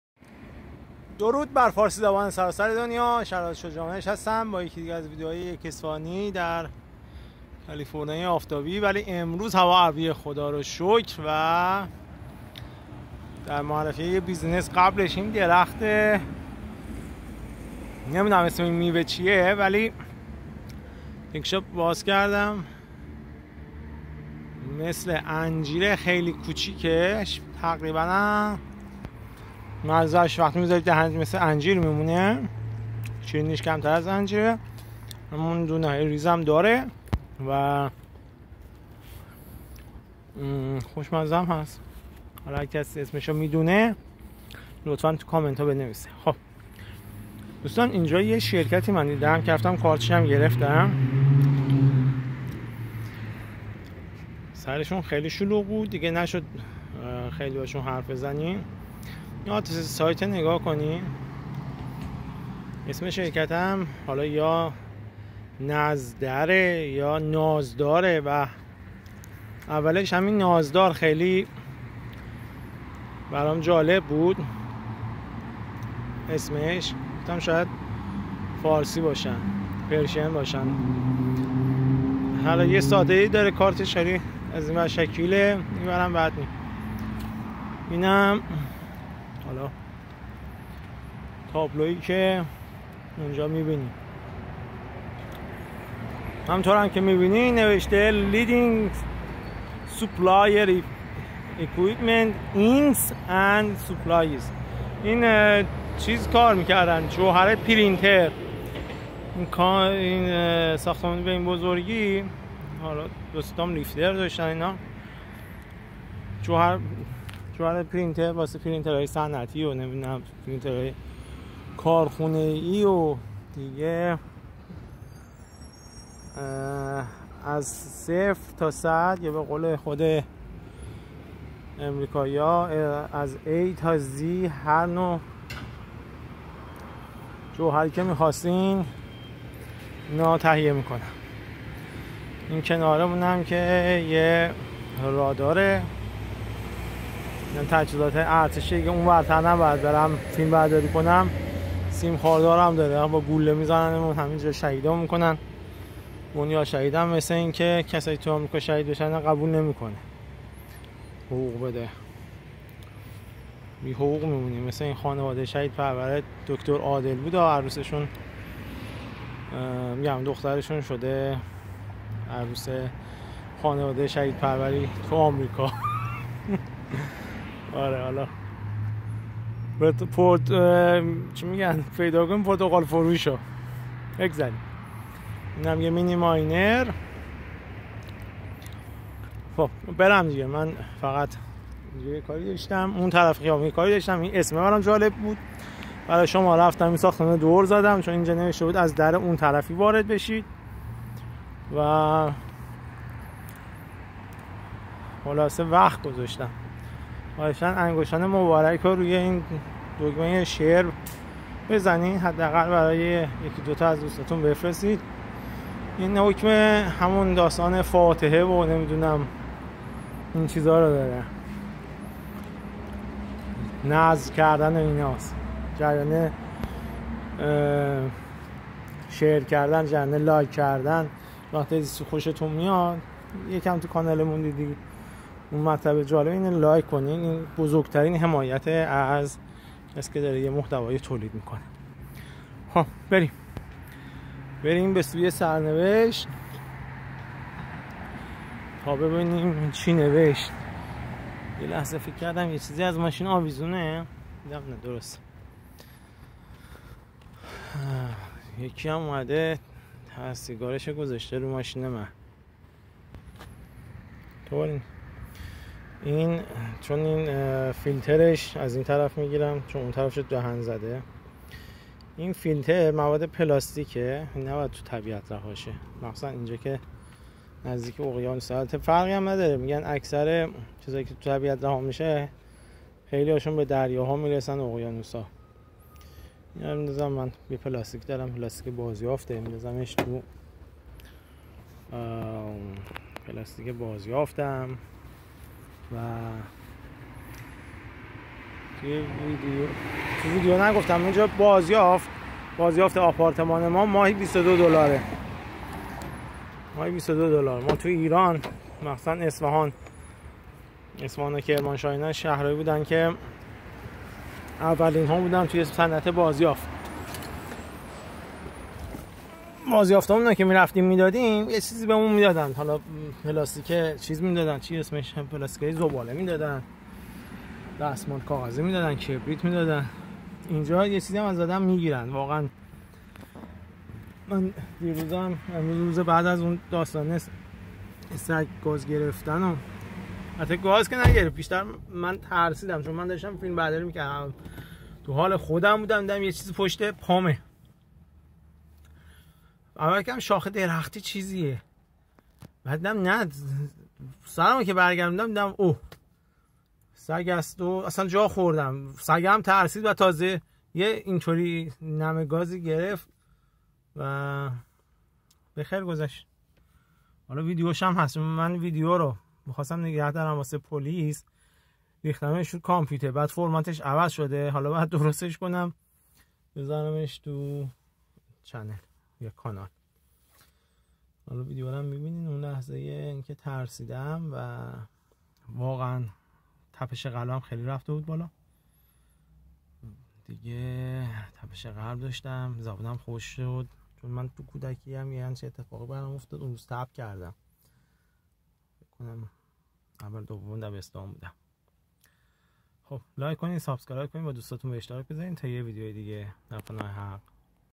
Nazdar نازدار
معرفی یک بیزینس شرکتی که فقط در زمینه جوهرهای پرینتر فعالیت دارد 👌